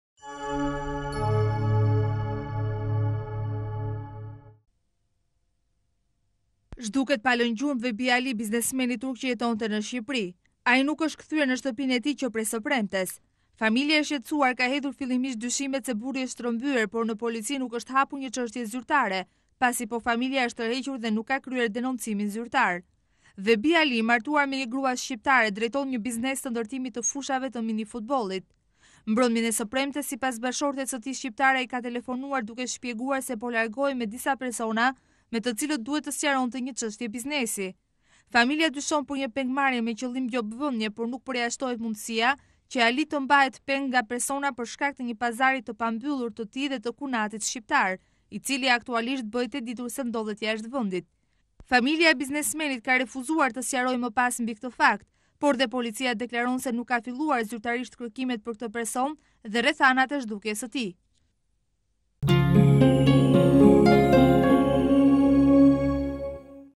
Zhduket biznesmeni turkt, Vebi Ali (13 Janar 2004)
Zhduket pa lënë gjurmë,  Vebi Ali, biznesmeni turk, që jetone në Shqipëri. Ai nuk është kthyer në shtëpinë e tij, që prej së premtes. Familja, e shqetësuar, ka hedhur fillimisht dyshimet se burri është rrëmbyer, por në polici nuk është hapur një çështje zyrtare, pasi po familja është tërhequr dhe nuk ka kryer denoncimin zyrtar . Vebi Ali, i martuar me një grua shqiptare, ndërton një biznes të ndërtimit të fushave të minifutbollit. Mbrëmjen e së premtes, sipas bashkëshortes së tij shqiptare, ai ka telefonuar, duke shpjeguar se po largohej me disa persona, me të cilët duhet të sqaronte një çështje biznesi. Familja dyshon për një pengmarrje me qëllim gjobëvënie, por nuk përjashtohet mundësia që Ali të mbahet peng nga persona, për shkak të një pazari të pambyllur të tij dhe të kunatit shqiptar, i cili aktualisht bëhet e ditur se ndodhet jashtë vendit. Familja e biznesmenit ka refuzuar më pas të sqarojë mbi këtë fakt, por edhe policia deklaron se nuk ka filluar zyrtairsht kërkimet për këtë person dhe rrethanat e zhdukjes së tij./tvklan.al